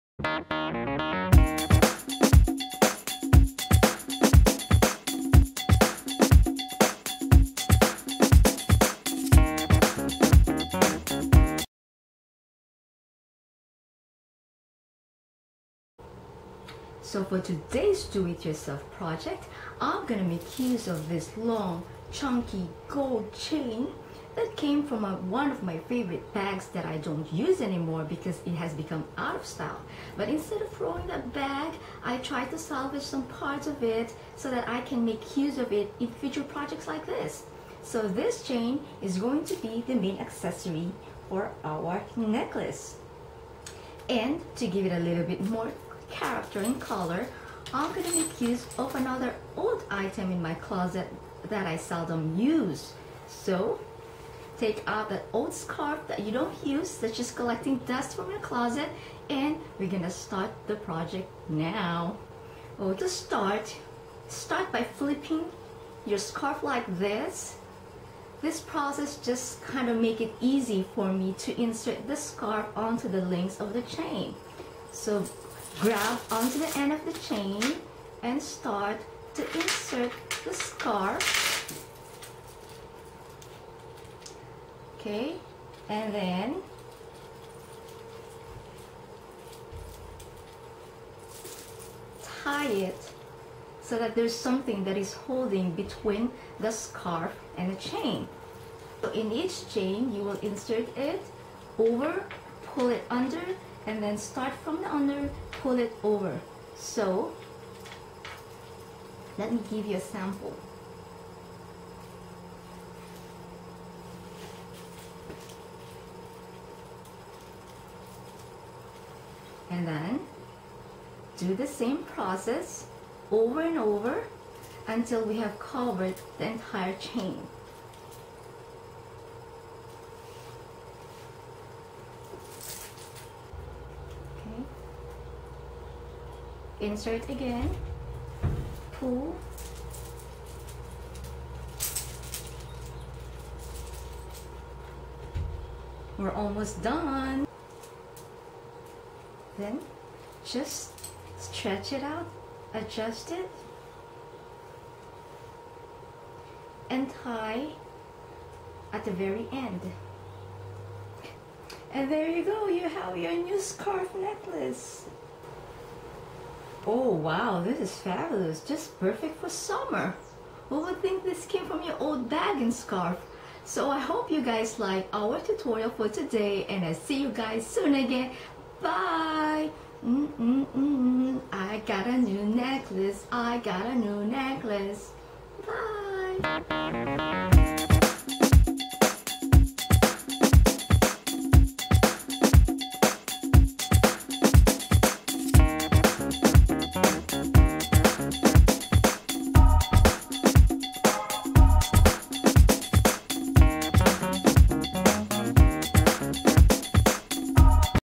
How to Make: FAB Chunky Necklace from Old Bag Chain & Scarf
Here's another handmade accessory! A chunky necklace made from an old bag chain (yes!) and a scarf that I seldom use. Watch the video to see how I transformed these two unlikely items to a stunning neckpiece that screams F-A-B for summer!